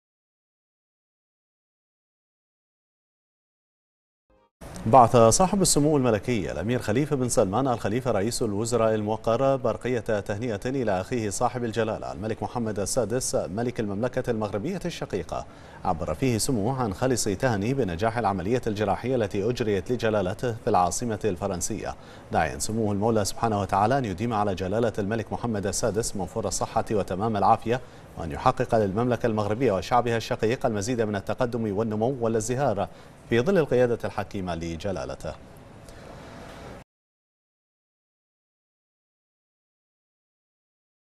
البحرين : سمو رئيس الوزراء يبعث برقية تهنئة الى اخيه جلالة العاهل المغربي
سمو رئيس الوزراء يبعث برقية تهنئة الى اخيه جلالة العاهل المغربي